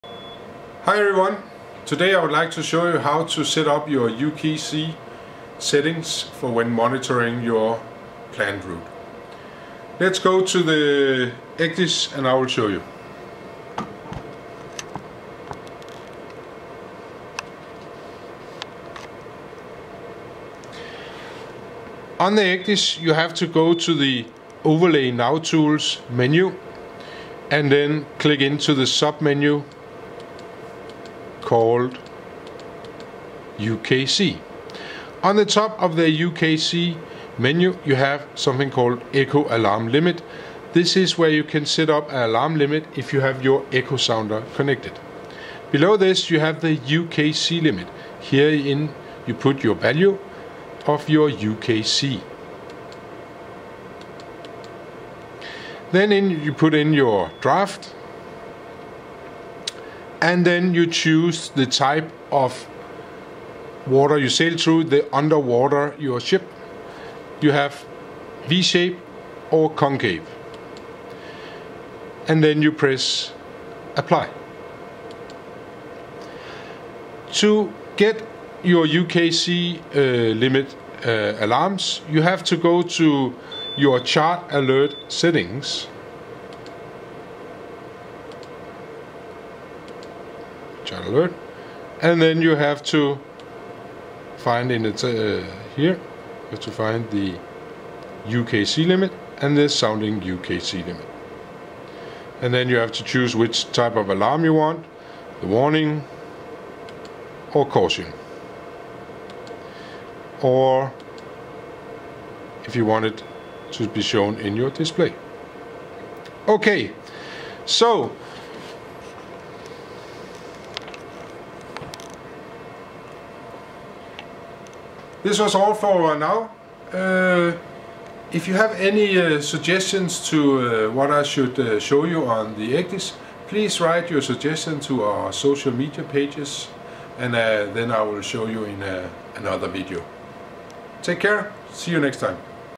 UKC setup for monitoring | FURUNO ECDIS tutorial
In this video, we will show you on the FURUNO ECDIS how to set up your UKC settings for monitoring your planned route.

Do you have any suggestions for new topics, please let us know in the commentary field.

Maritime training for professionals!